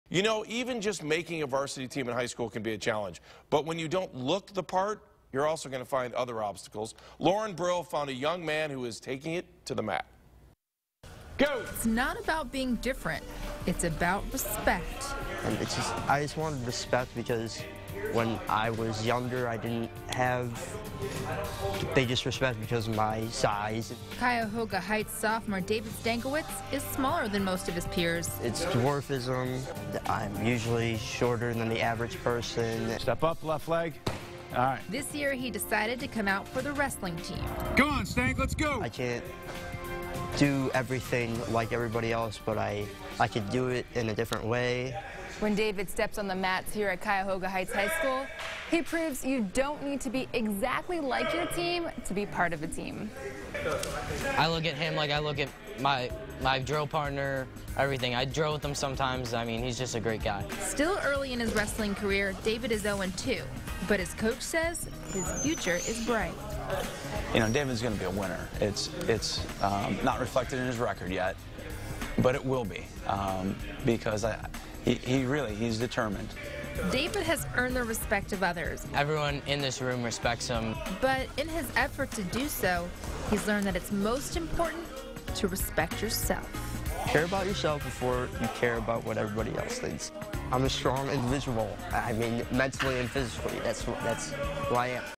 Wrestling for respect
Cuyahoga Heights wrestler is motivated by respect.